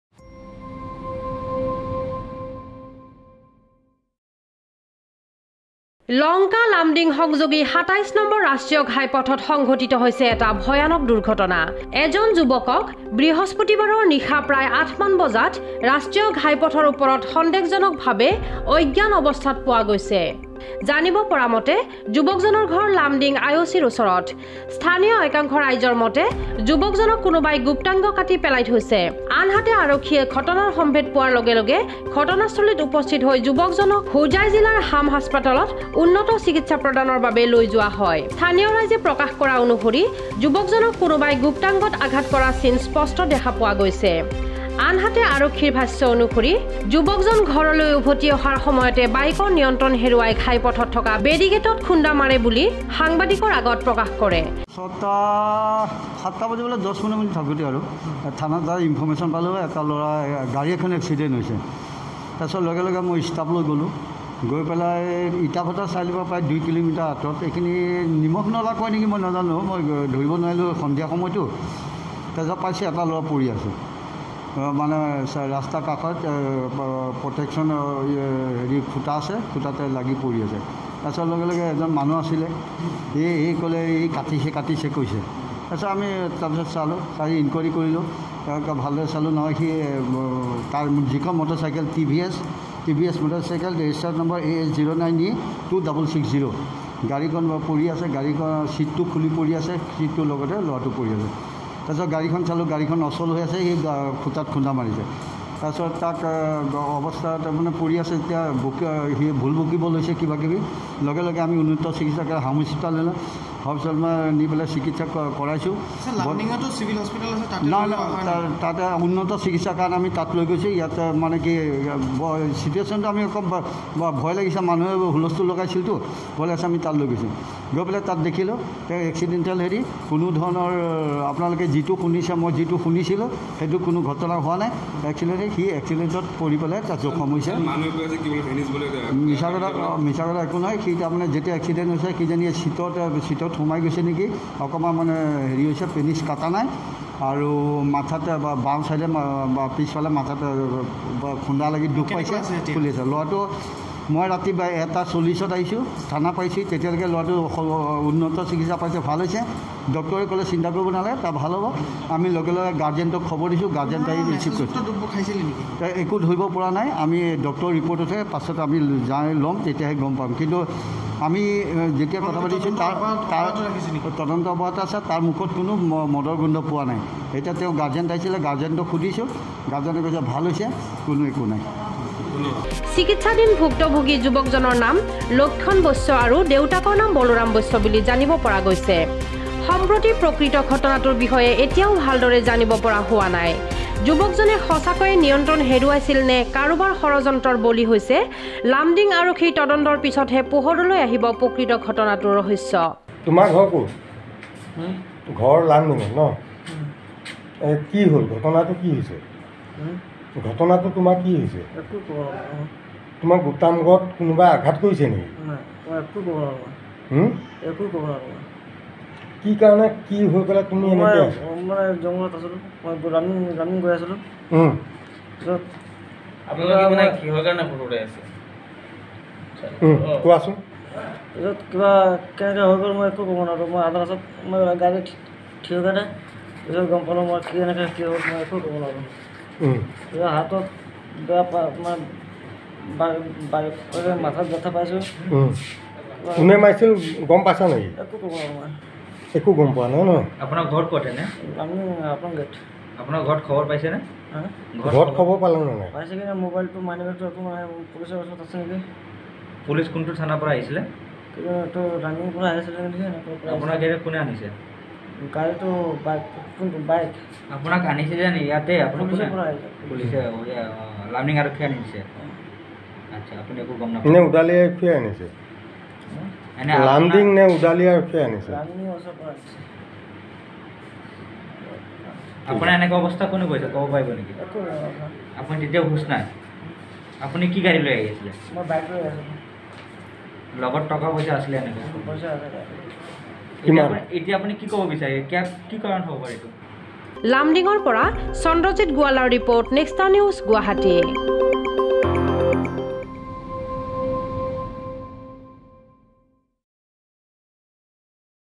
A young man rescuing on road in an unconscious stage.
যোৱা ‌ৰাতি ৮ বজাত লংকা লামডিং সংযোগী ২৭ নং ৰাষ্ট্ৰীয় ঘাইপথত সন্দেহ জনক  অজ্ঞান ‌অৱস্থাত এজন যুবক‌ ঘাইপথত উদ্ধাৰ হোৱা ঘটনাই চাঞ্চল্য়ৰ সৃষ্টি কৰিছে অঞ্চলজুৰি ।